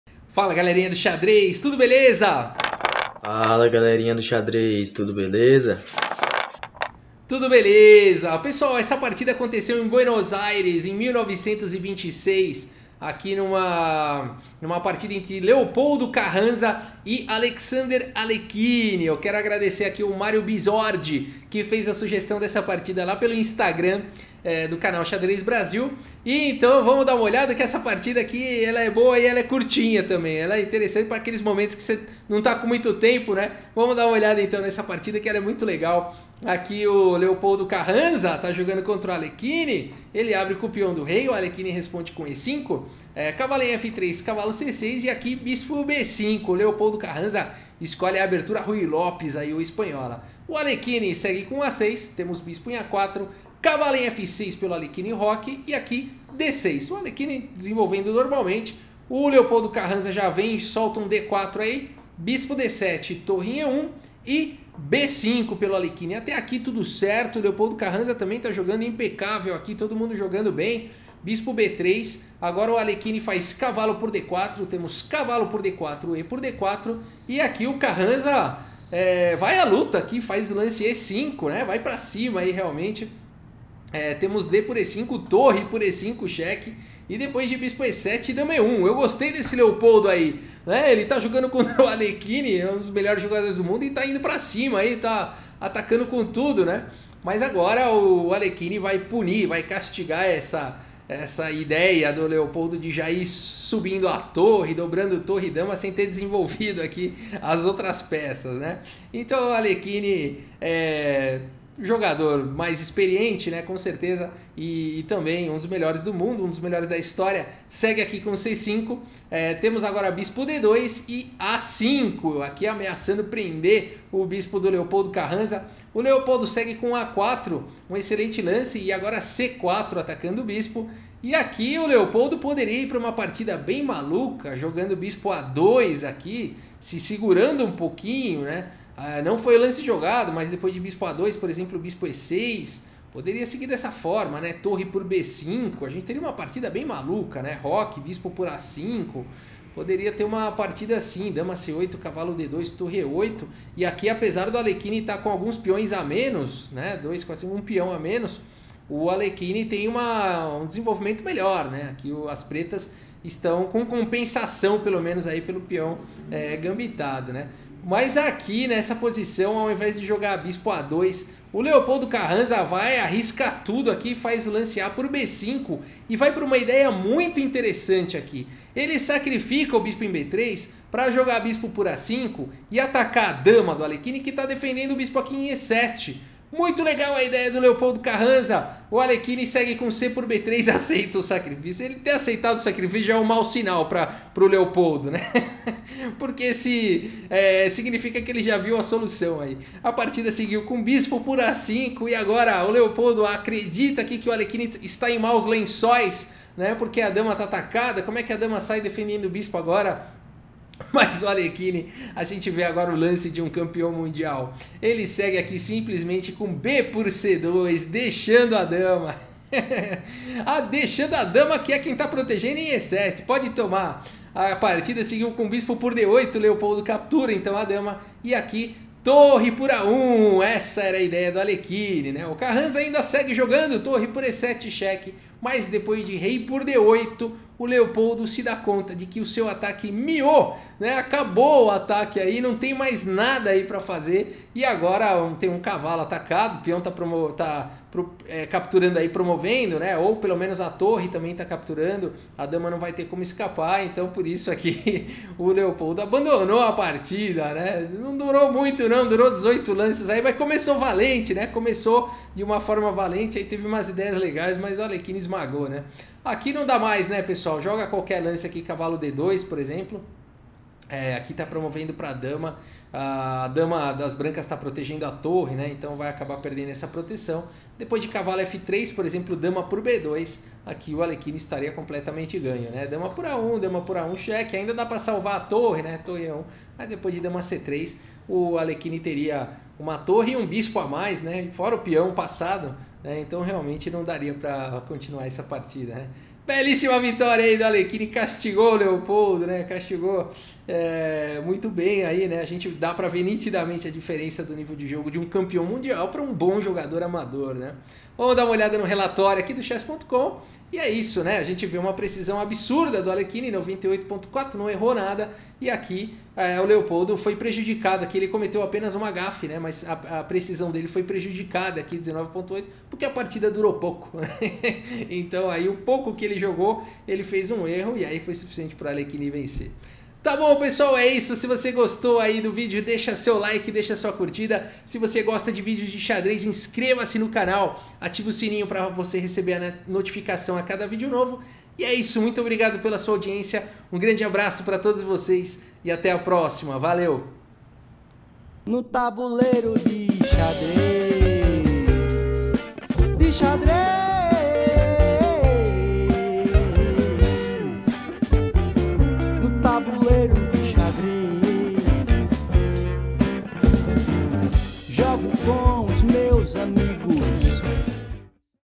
Querendo AGREDIR o campeão MUNDIAL de xadrez? || Leopoldo Carranza x Alexander Alekhine (1926)
Nesta emocionante partida de xadrez entre Leopoldo Carranza e Alexander Alekhine em 1926, testemunhe um confronto intenso enquanto Carranza busca desafiar o campeão mundial. Com movimentos estratégicos e jogadas ousadas, este jogo histórico cativará os entusiastas do xadrez. Assista agora e descubra se Carranza conseguirá agredir o campeão mundial nesta partida memorável.

### MELHORE NO XADREZ ###
👉 Conheça Nossos Cursos de Aberturas, Cálculo, Táticas, Estratégia e Finais

Melhore no Xadrez na Escola Xadrez Brasil, aulas com GM André Diamant 2 vezes por semana